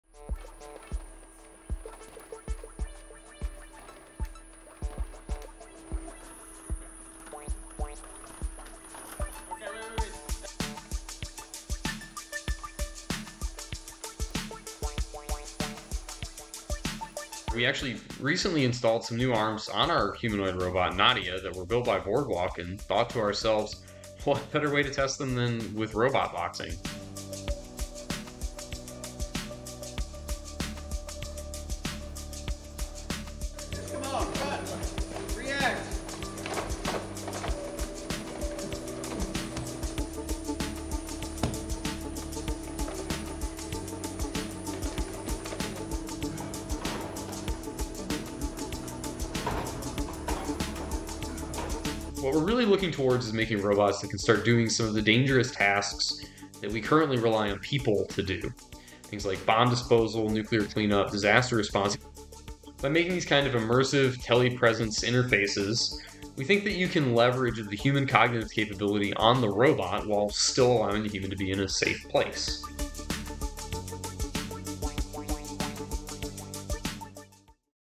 Robot packs a punch against human opponent | Reuters
Going toe-to-toe in the boxing ring is the perfect way to test humanoid robot Nadia, according to researchers at the Institute for Human & Machine Cognition in Pensacola, Florida.